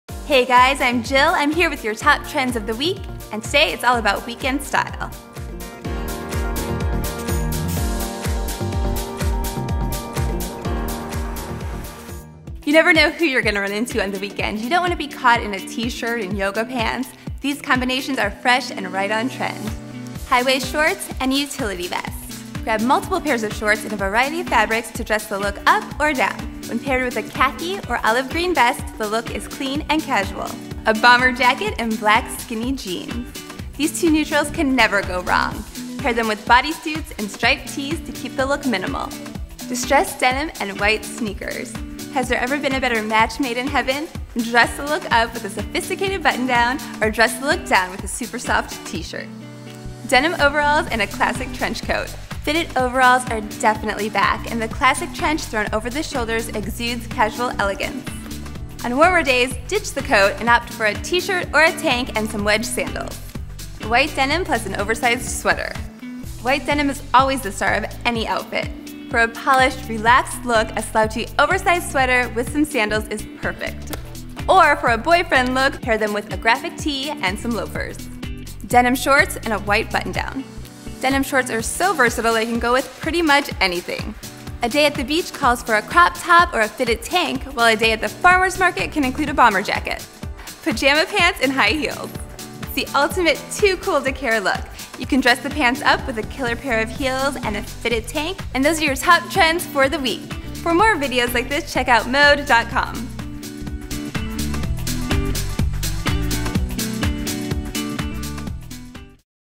Weekend Style | What’s on Trend ★ Glam.com
Mode’s own Jill Manoff takes you through the top style trends of the week. Check out the Glam editor’s top style pairings for the weekend, including what to wear for a polished look and what will work for a Memorial Day beach vacation. You'll also learn how to dress the looks up and down for day and night.